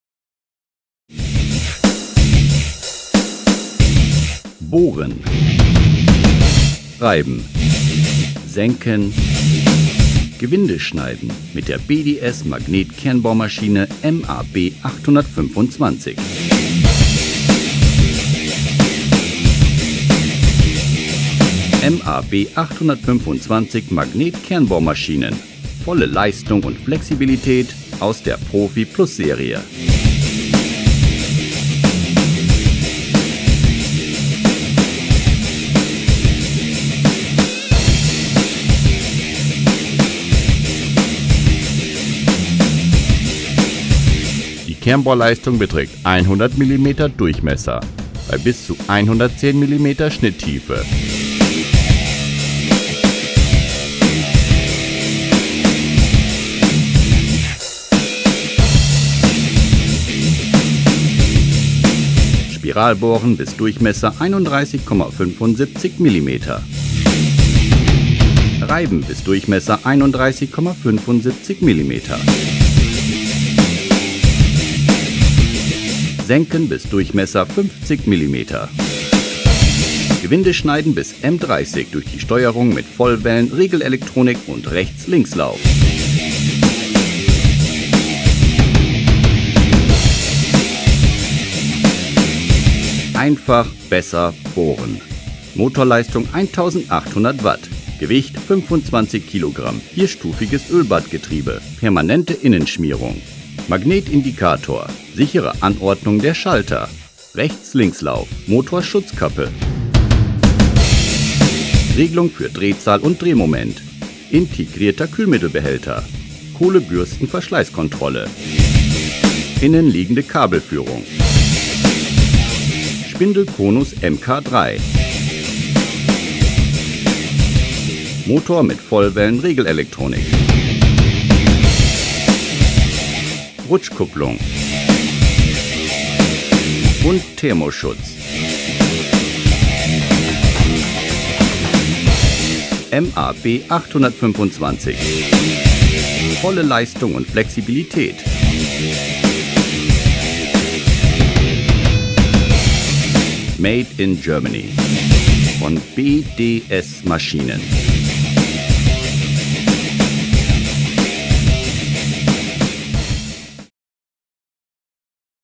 Die beste Magnet-Kernbohrmaschine MAB 825 | MK3, ⌀ 100 mm Kernbohren & M30 Gewinde schneiden
Die Magnetkernbohr Maschinen des ProfiPLUS-Programms liefern das komplette Leistungsspektrum für Kernbohrungen, Spiralbohrungen, Senken und Gewinde schneiden. Möglich wird dies durch die Ausstattung der Magnetkernbohr Maschinen mit „reversiblen Motoren“. Diese sind sowohl für Rechts- als auch für Linkslauf geeignet. BDS Maschinen eröffnet als einer von wenigen Anbietern weltweit die Möglichkeit dieses universellen Einsatzes. Made in Germany.

MAB 825 Magnetkernbohr Maschine.
100 mm Bohrleistung, M30 Gewindeschneidleistung, 1800 Watt Motorleistung, 25 Kg Gewicht.